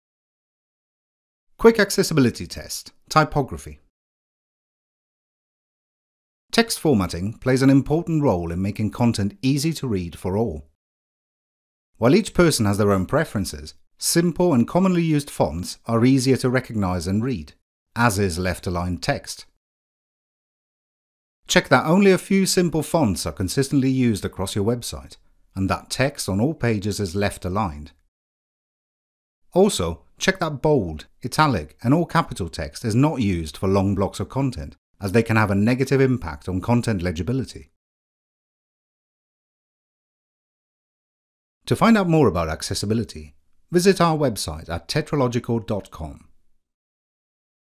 Quick accessibility test: Typography (British Sign Language version)
Learn how to test typography on your website. For a full text transcript of the video select "Show more" below.

Text transcript:

Quick accessibility test: Typography

Text formatting plays an important role in making content easy to read for all. While each person has their own preferences, simple and commonly used fonts are easier to recognise and read, as is left-aligned text.

Check that only a few simple fonts are consistently used across your website, and that text on all pages is left-aligned.

A page from the TetraLogical website where a simple font is used and all text is left aligned.

Also, check that bold, italic and all-capital text is not used for long blocks of content, as they can have a negative impact on content legibility.

The page from the TeatraLogical website does not contain any bold, italic or all-capital text.